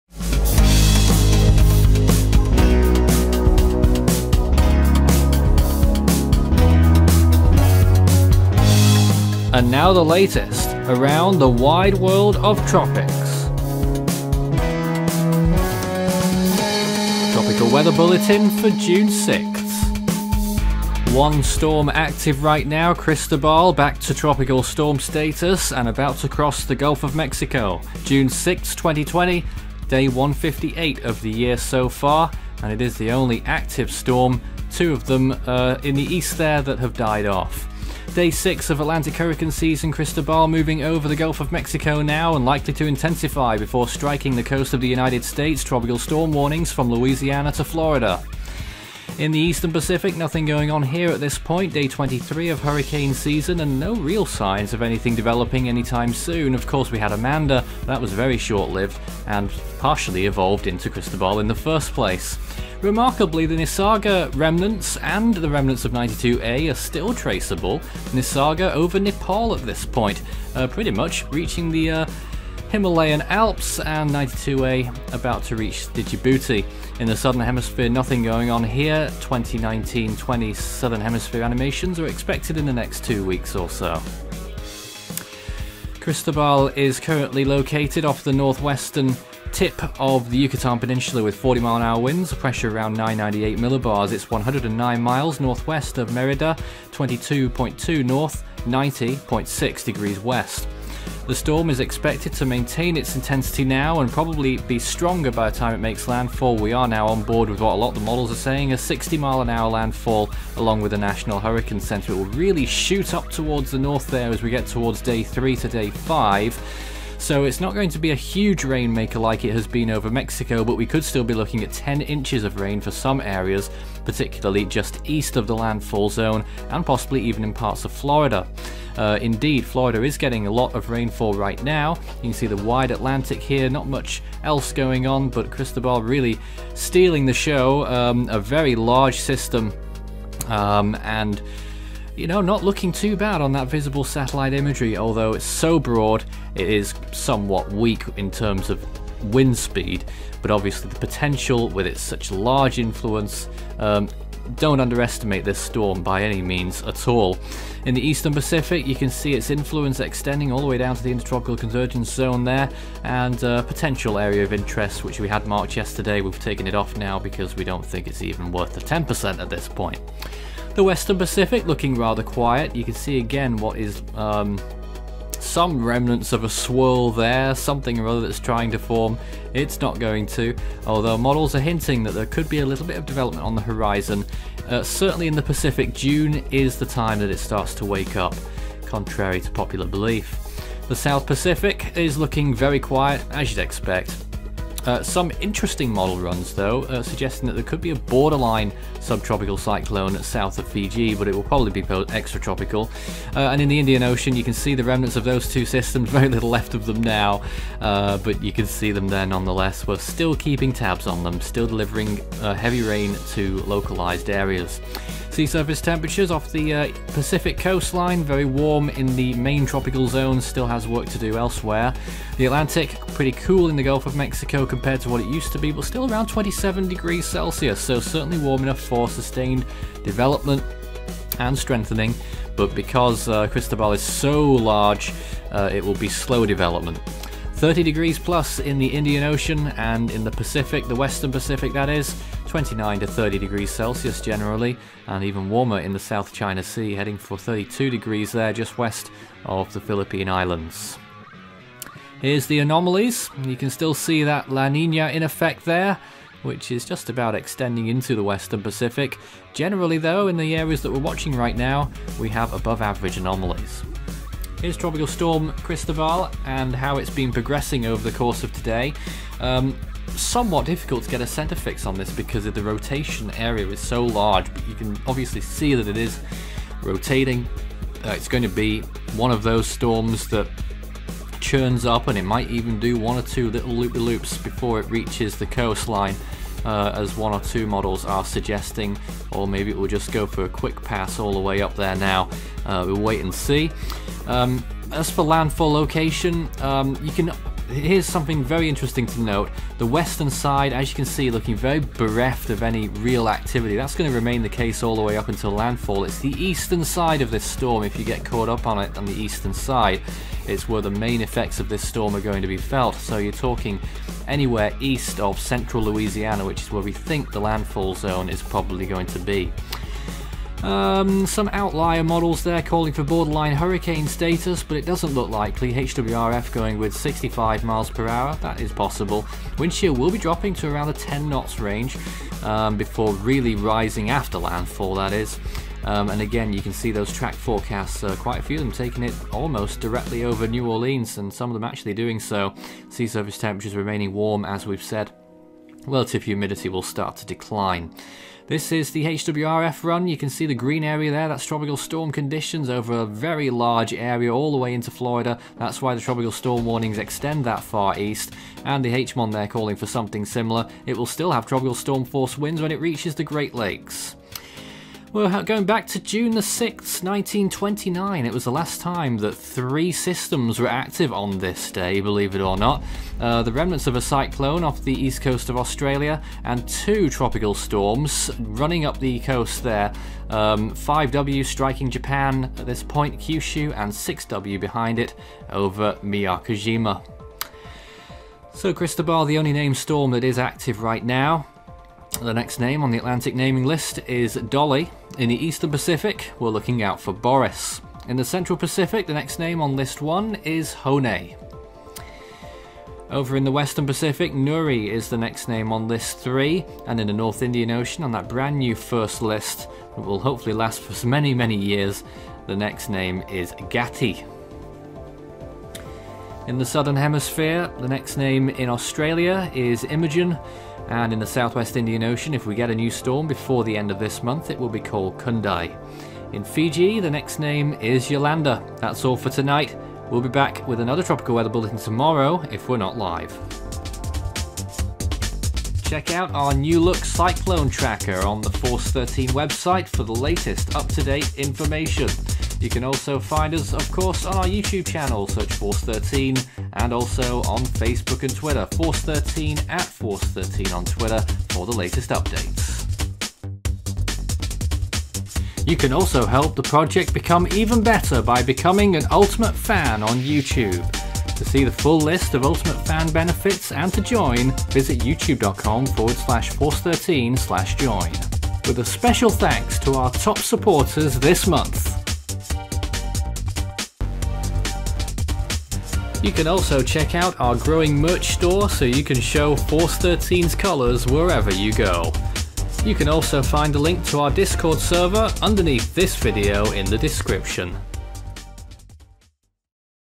Tropical Weather Bulletin - Tropical Storm Cristobal - June 6, 2020
Tropical Storm Cristobal made landfall in Tabasco and Campeche in Mexico on Wednesday with winds near 60mph, and has now emerged over the Gulf of Mexico, near the Yucatan Peninsula. Earlier today it restrengthened back to storm status, and will likely strengthen further and strike Louisiana with winds near 60mph.
Due to Cristobal's size, tropical storm conditions could be felt as far east as the Florida panhandle, with up to 10 inches of rain possible over a widespread area on the eastern side of the storm.